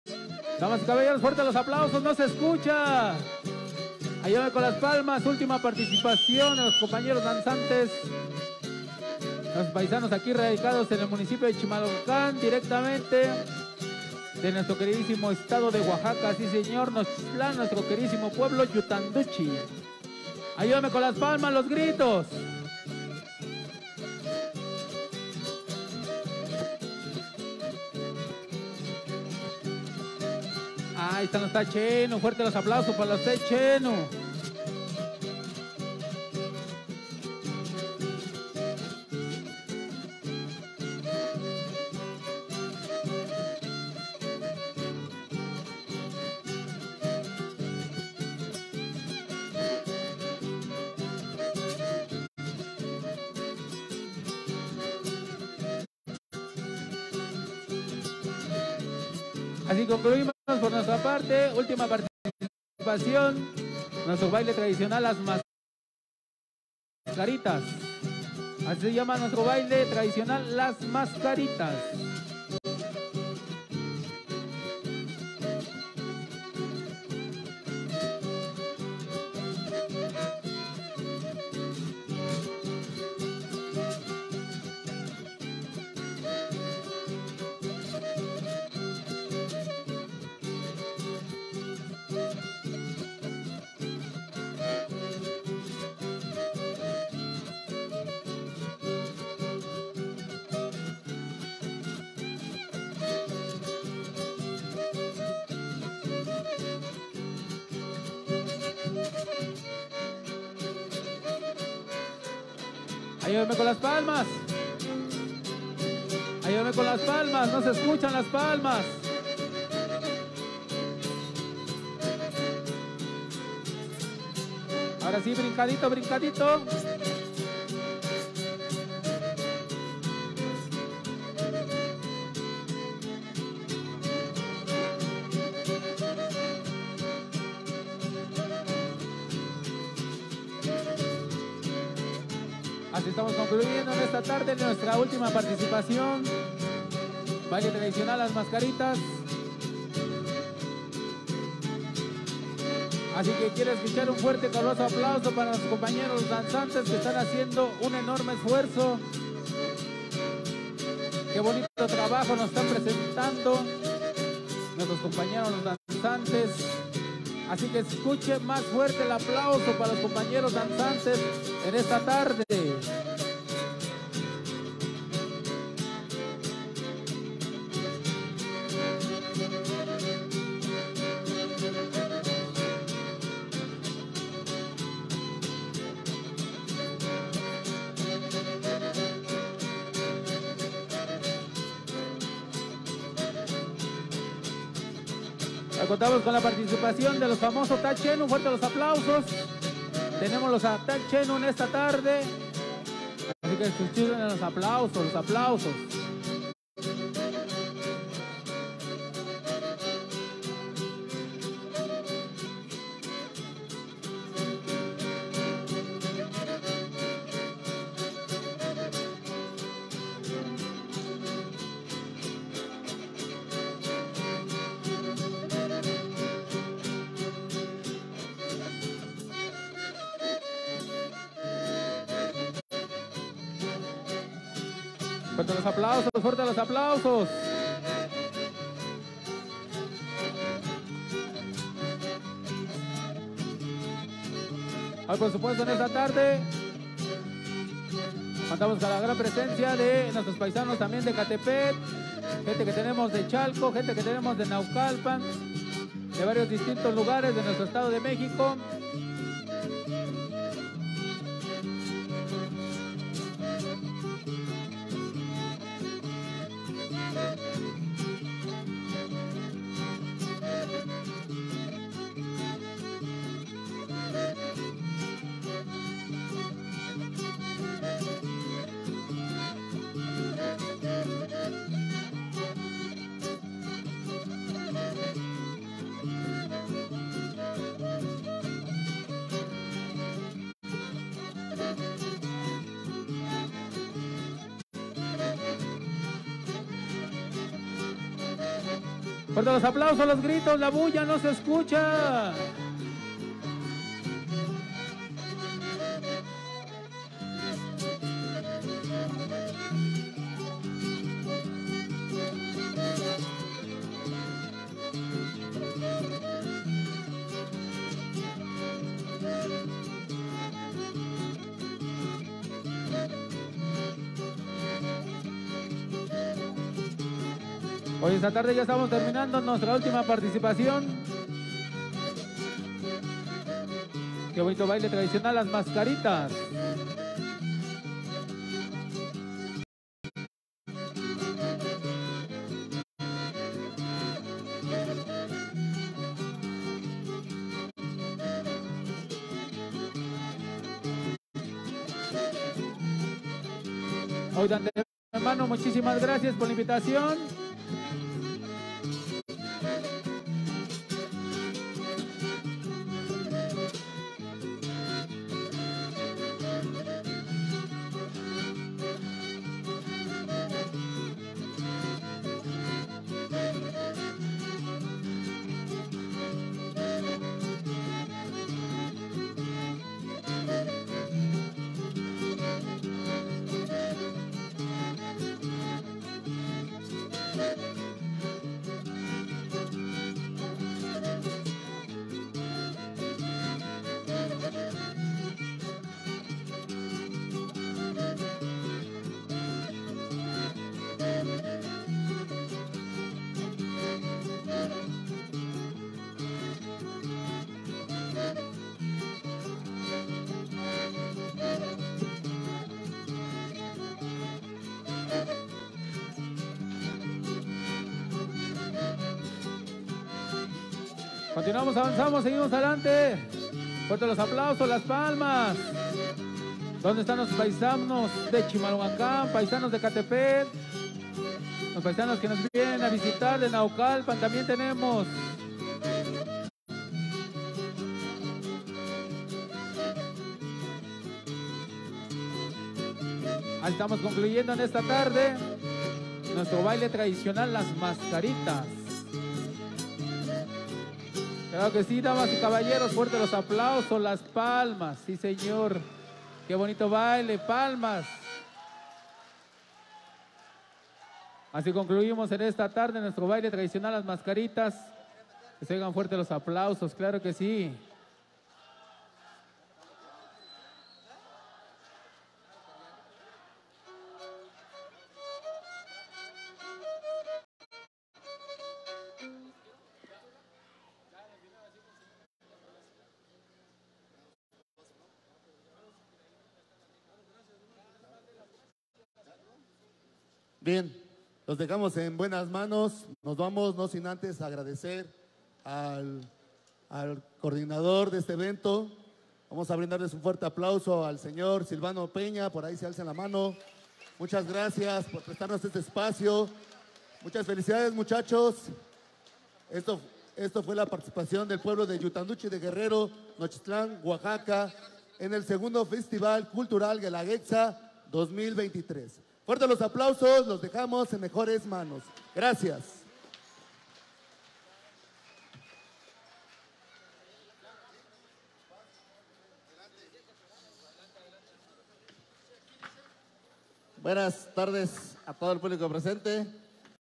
Mascaritas YDG en el 2do festival cultural oaxaqueño Chimalhuacán Guelaguetza 2023
dando a conocer la tradicion del baile de las mascaritas de yutanduchi de guerrero nochixtlan oaxaca. radicados en el municipio de chimalhuacan